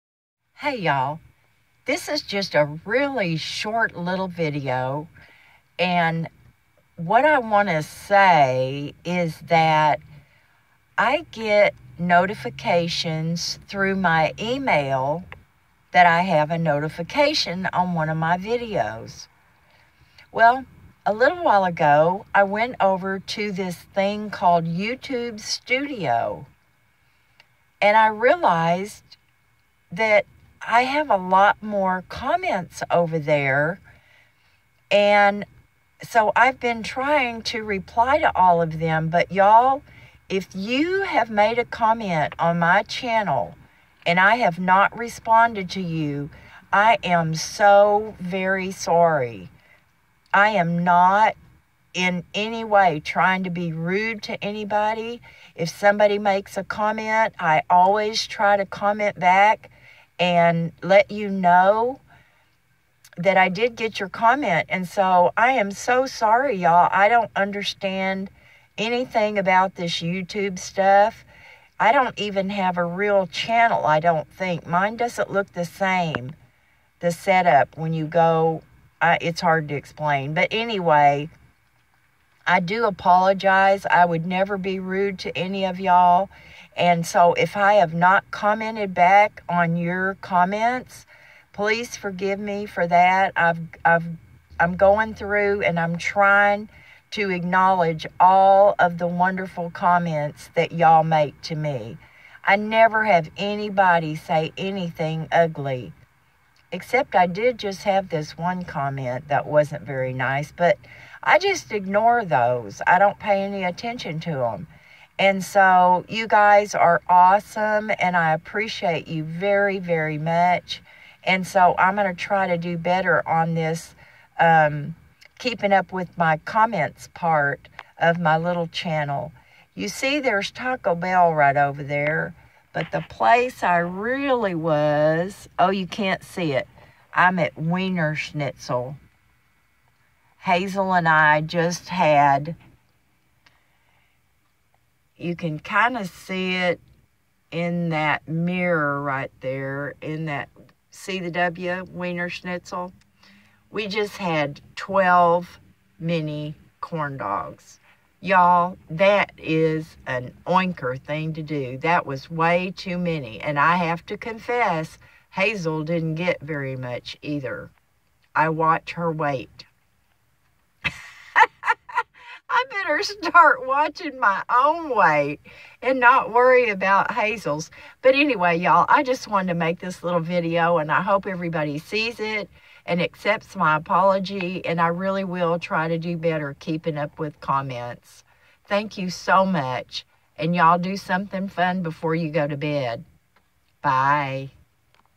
Oh my gosh!! I did not know!
I did not realize that there was another place that I could look at all my comments!! I get notifications on comments through my email but that's not nearly all of them, so I really am so sorry you guys, I would never ignore anybody!